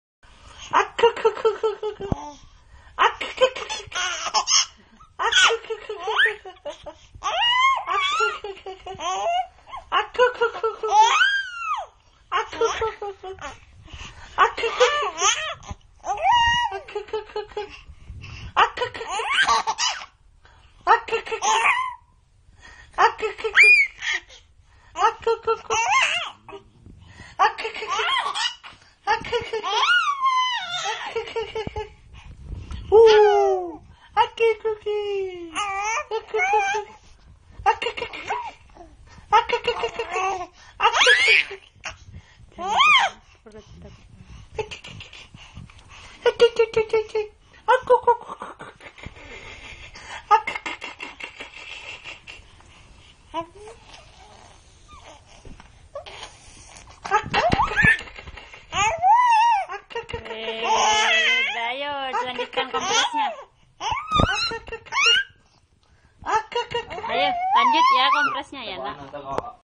Kenzo bercanda ma ayah
Kenzo bercanda ma ayah tuhfah di kamar